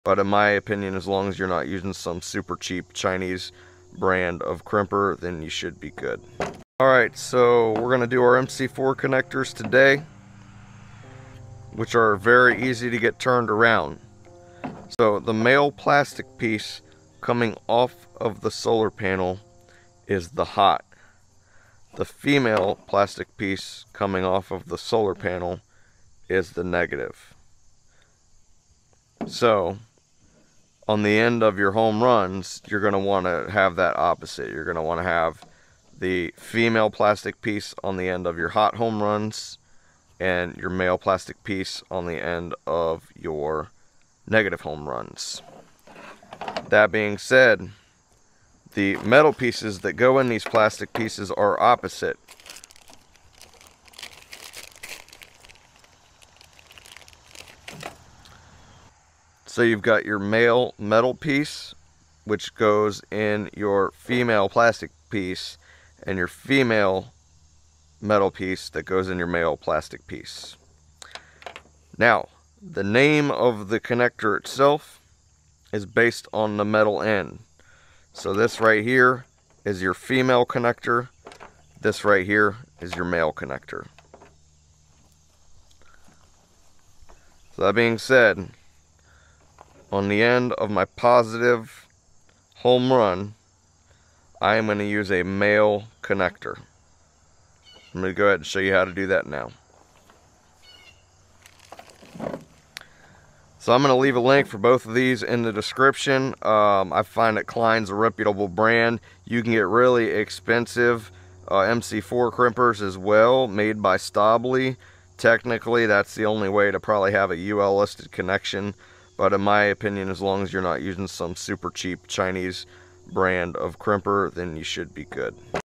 MC4 Connector Polarity: Wire Your Solar Panels Like a Pro!
Confused about MC4 connectors? Many people accidentally reverse polarity when wiring solar panels. This video explains how to correctly connect MC4s, ensuring positive and negative polarities are right for safe and efficient solar energy.